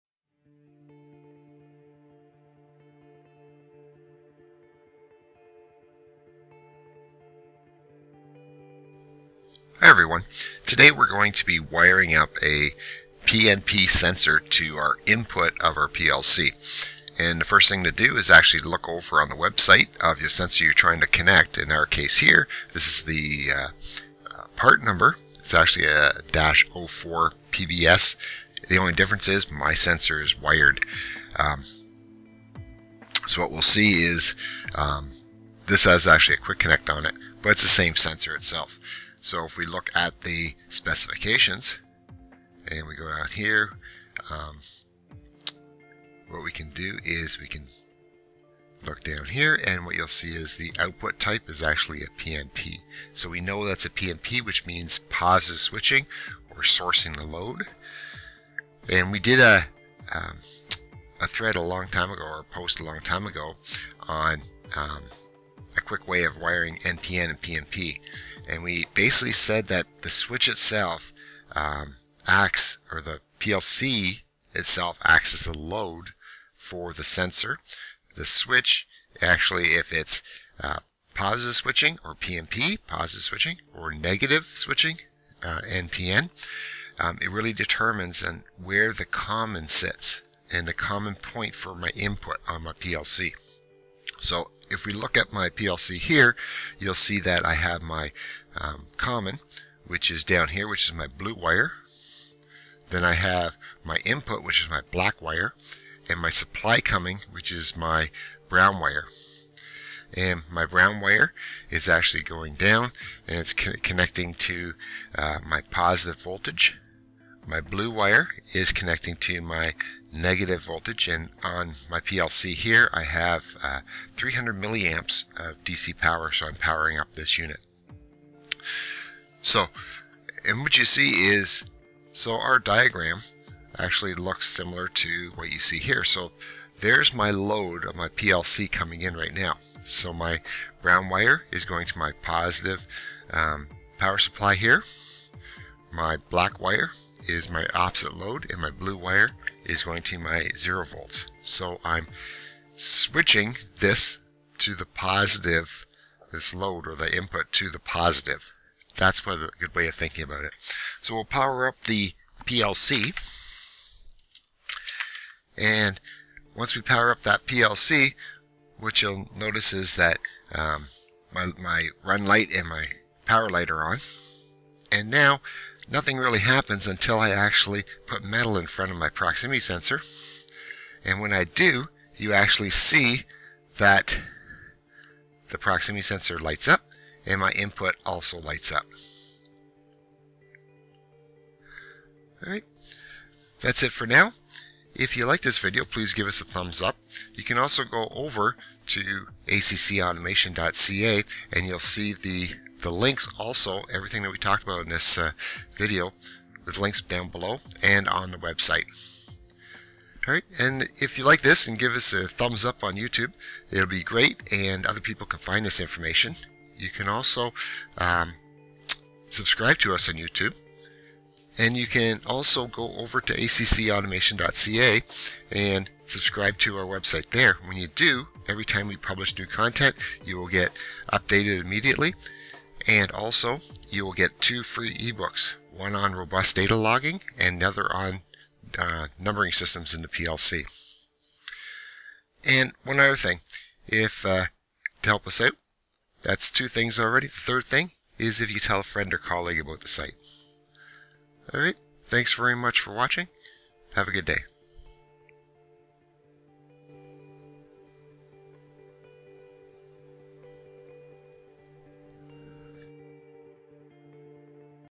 Connect Your PNP Sensor to PLC Like a Pro!
PNP wiring to plc can be confusing. We will show the wiring of a PNP sensor into a PLC. The proximity sensor is Sick. Here is a link to the specifications.
The PLC is an Omron CP1H. Here is a link for additional information on the PLC.

00:00 Wiring PNP Sensor
00:24 PNP sensor specification
01:10 PNP positive switch wiring
02:13 PNP wiring diagram
02:53 Operation of PNP sensor to Omron PLC

Wiring a three-wire DC sensor is a matter of determining what kind of output you have. The two types are NPN and PNP.
NPN is referred to as sinking the load or negative switching.
PNP is referred to as sourcing the load or positive switching.
In the video, we wire up a PNP proximity sensor. The load for the proximity sensor is the input of the PLC. We switch the load to the positive side, so that means that the common on the PLC sits at 0VDC.
PNP sensor has three wires:
Brown - +24VDC
Black - Switching Wire
Blue - 0VDC
In our case, the PLC load appears between the input point (Black) and common (Blue). The positive voltage (Brown) is connected to +24VDC.

Regards,
Garry
ACC Automation